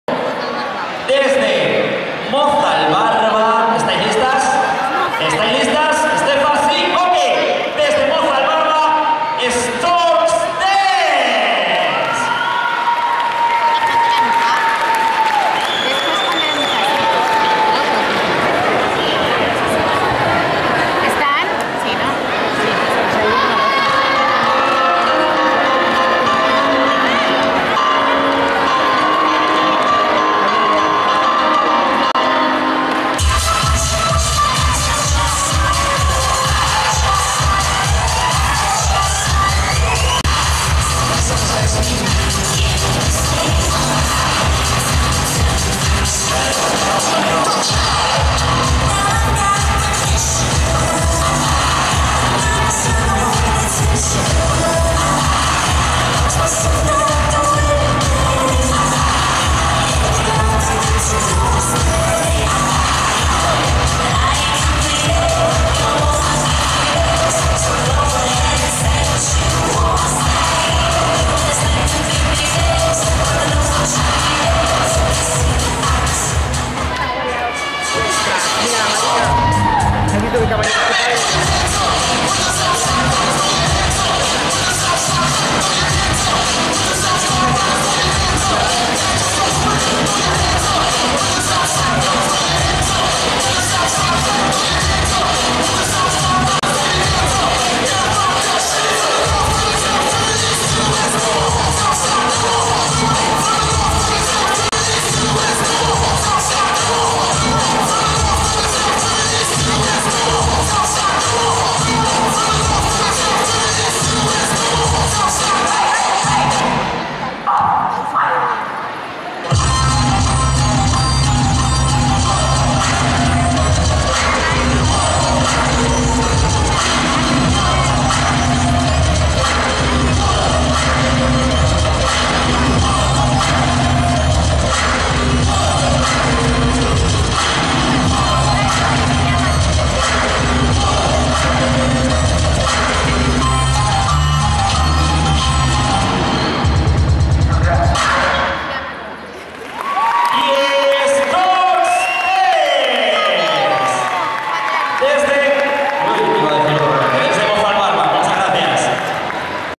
Exhibición de Navidad 2011 Utebo Storks Dance.
Exhibicion realizada en el palacio de los deportes de Utebo, organizada por el Espacio Joven de Utebo Zaragoza